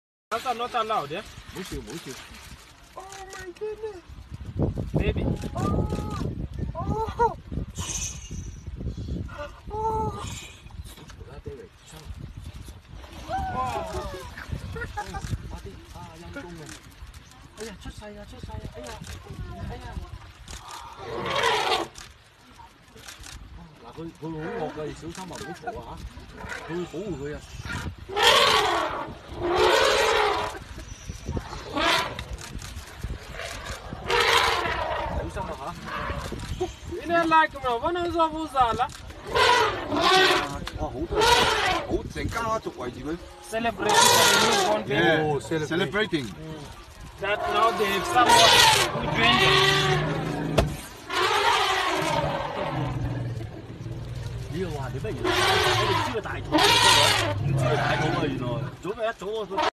GIVING BIRTH OF AN ELEPHANT
When the baby elephant is delivered, the sister elephants do two things: they kick sand or dirt over the newborn to protect its fragile skin from the sun, then start trumpeting, a female celebration of new life.